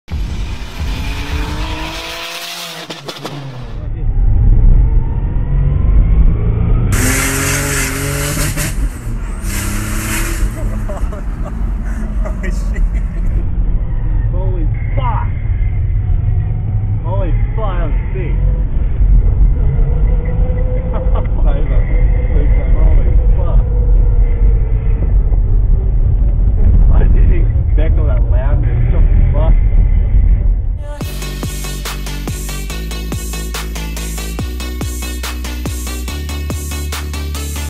TY's FIRST TASTE OF BOOST! - VR V6 TURBO COMMODORE UTE
Ty finally got to experience a small hit on low boost in the crazy daily driven custom turbo VR V6 commodore ute!